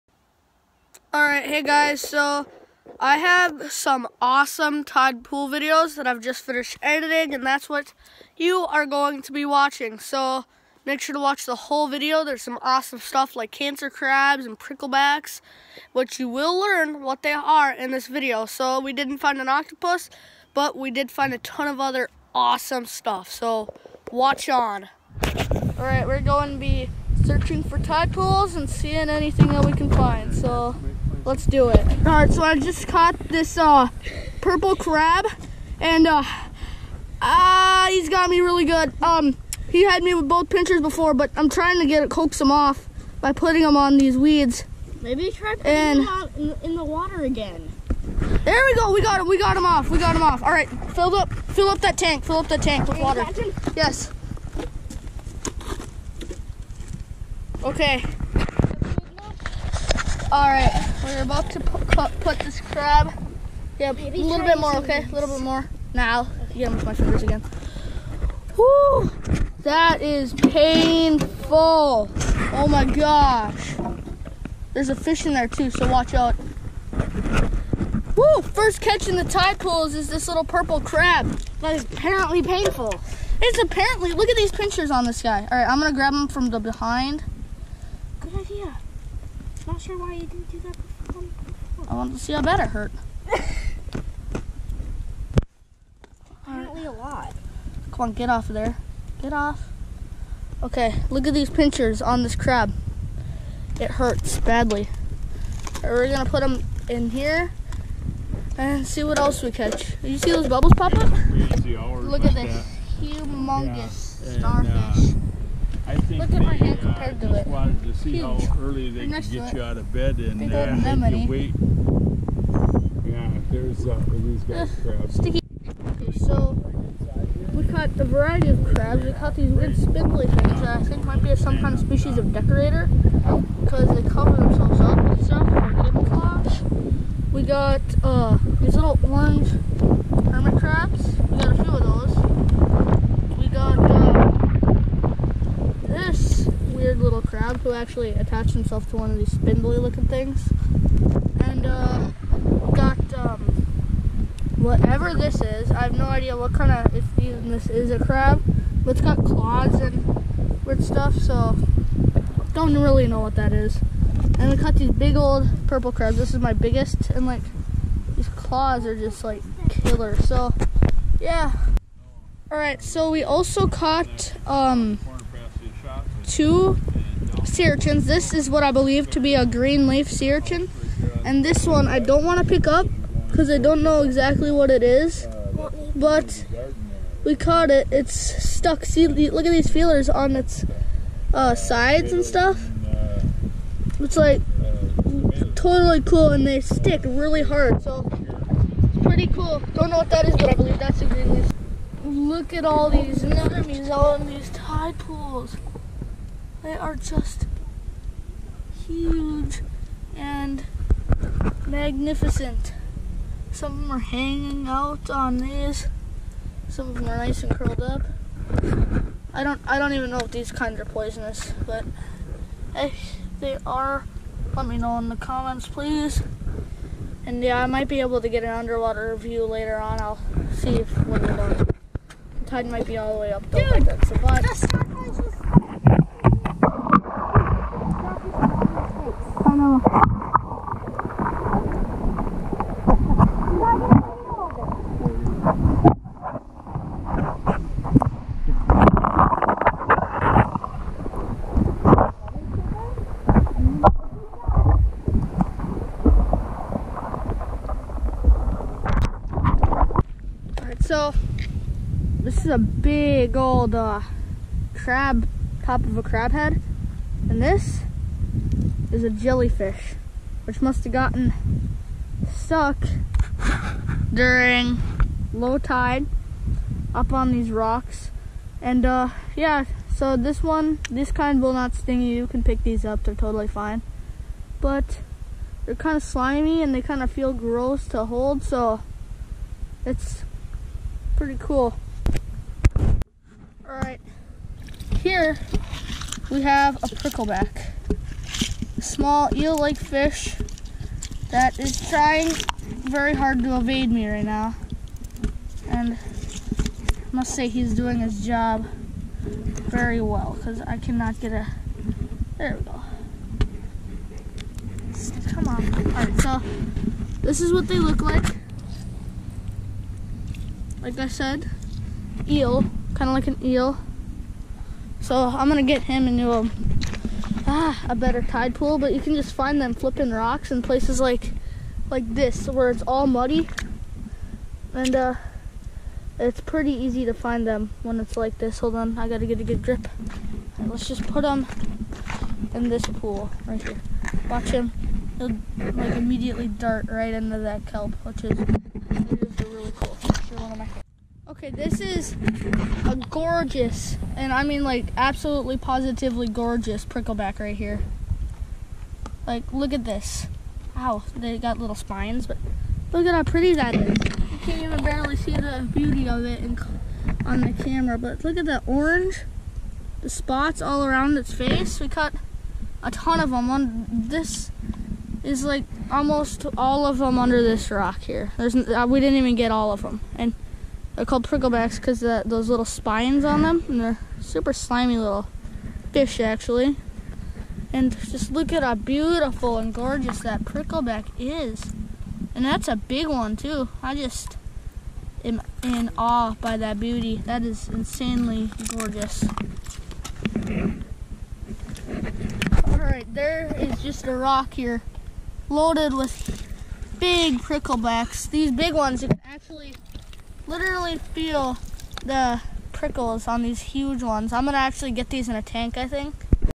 Exploring the Tide Pools of ALASKA! (Decorator Crabs, Pricklebacks, and much more!)
This was incredible and we caught many things! We caught the eel-like pricklebacks, decorator crabs, cancer crabs, anemones, hermit crabs, sea urchins, and more!